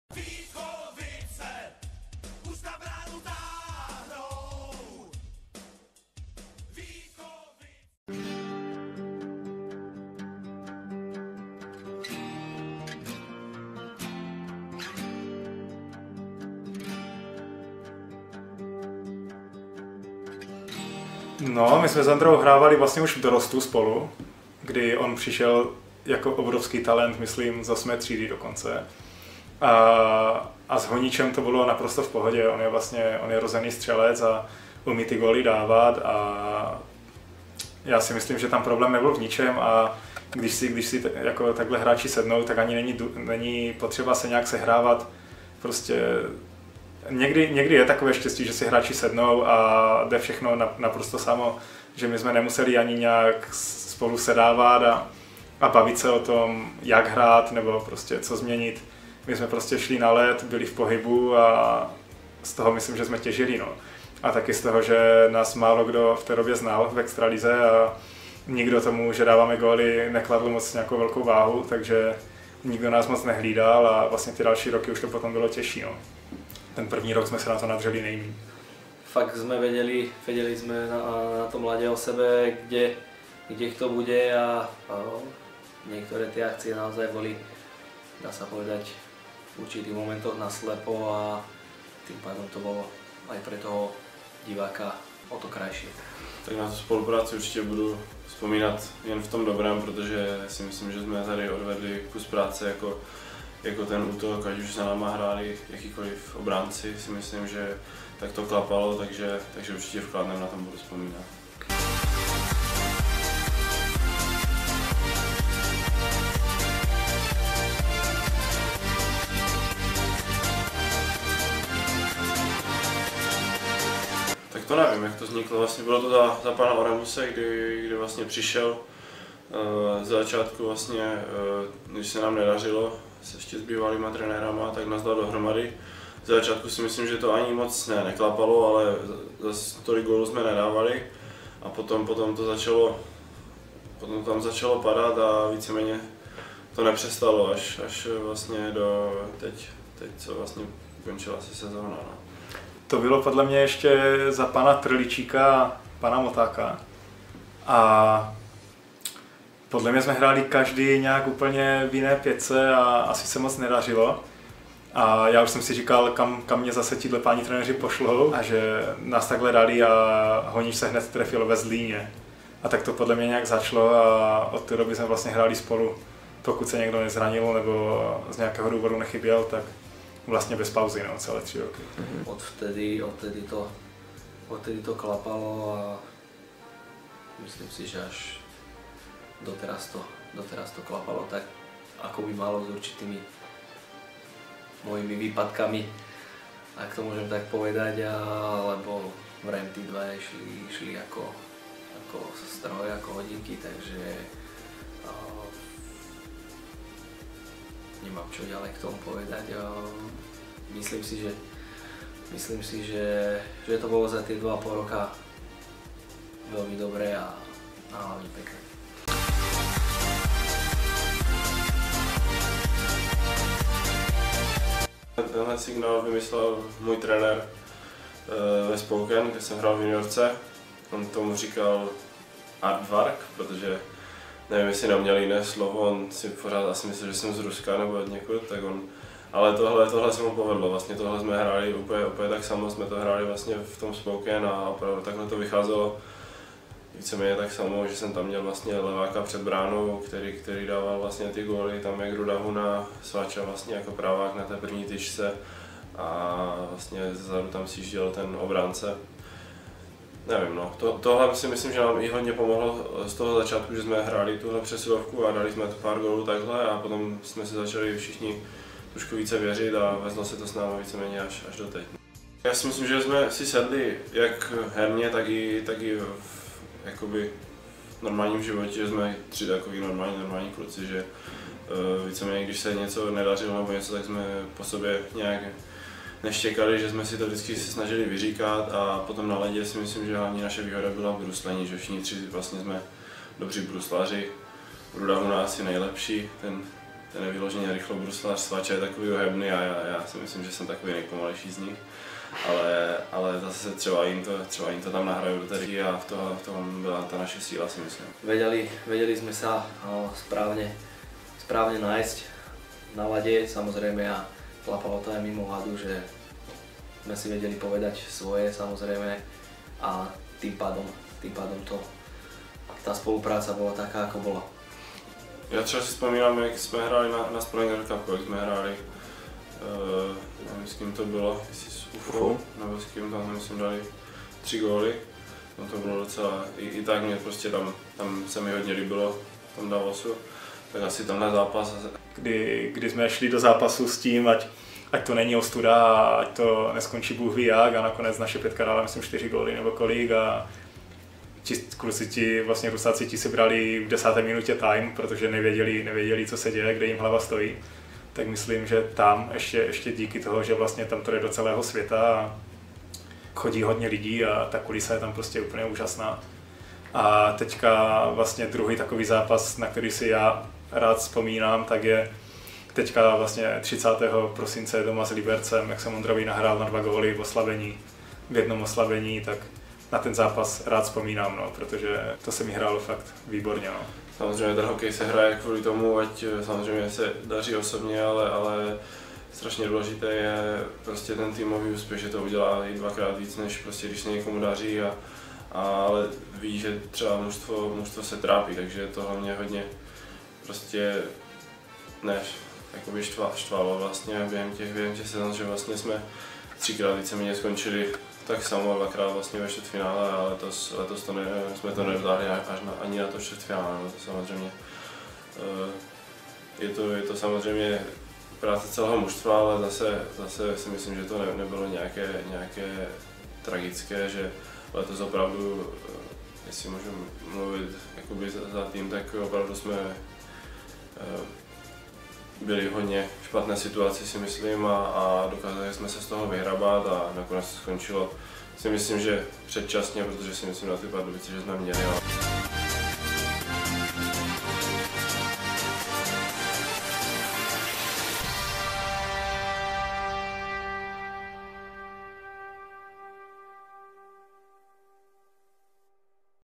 Romanovci - nejproduktivnější útok Vítkovic 2012 - 2015
Ohlédnutí za úspěšnými dvěma a půl sezonami útočné formace HC VÍTKOVICE STEEL Vladimír Svačina - Ondřej Roman - Rudolf Huna